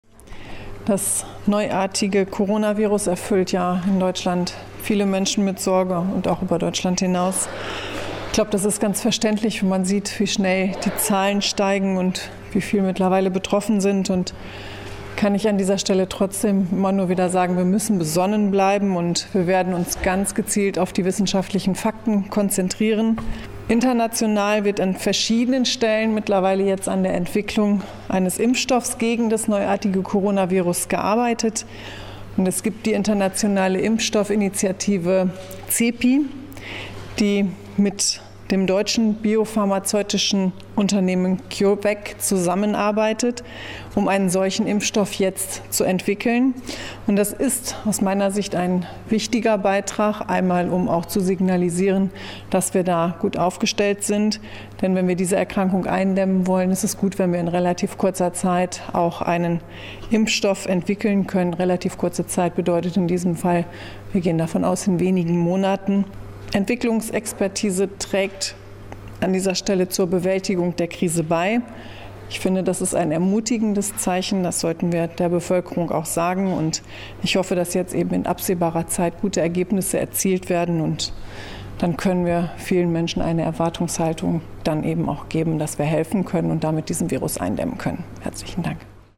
Forschungsministerin hofft auf baldigen Corona-Impfstoff
Laut Ministerin Anja Karliczek ist die internationale Impfstoffinitiative CEPI gut aufgestellt, um in wenigen Monaten einen Impfstoff entwickeln zu können.